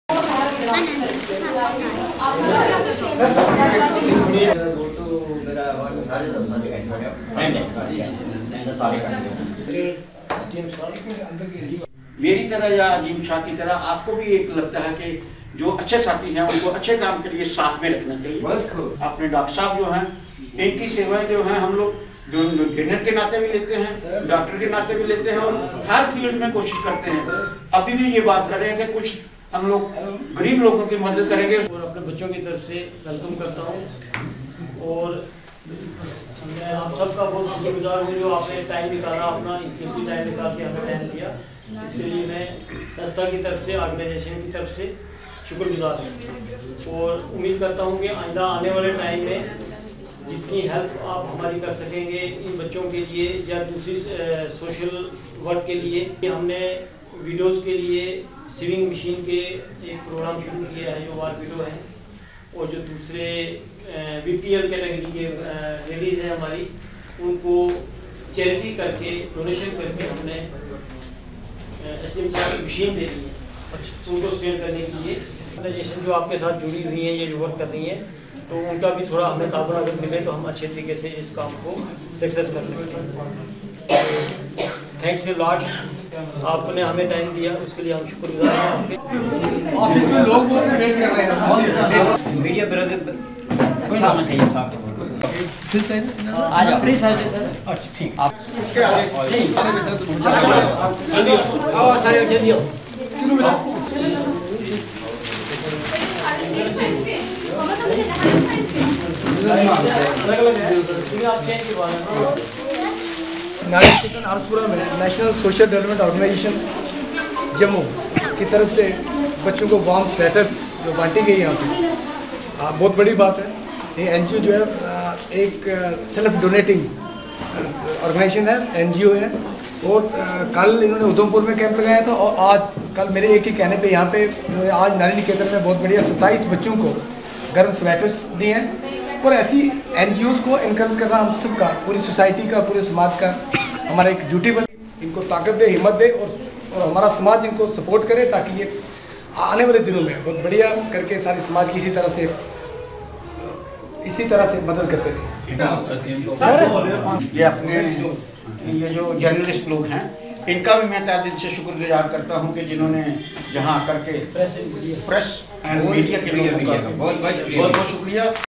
NATIONAL SOCIAL DEVELOPMENT ORGANISATION MENDHAR JAMMU
NATIONAL SOCIAL DEVELOPMENT ORGANISATION  MENDHAR JAMMU KASHMIR DISTRIBUTED WARM SWEATER IN NARI NIKETAN R.S.PURA JAMMU UNDER THE CHAIRMANSHIP OF SDM R.S.PURA NARESH KUMAR SHARMA- KAS . The program was organised in collaboration with Shri Kasturi Lal secretary Gandhi Seva Centre Senior consultant ircs Jammu Dr M L  Sharma Hemant Sharma, Rahul Kumar joint secretary and madam LATIKA Sharma  SUPDT Nari Niketan also present on the occasion.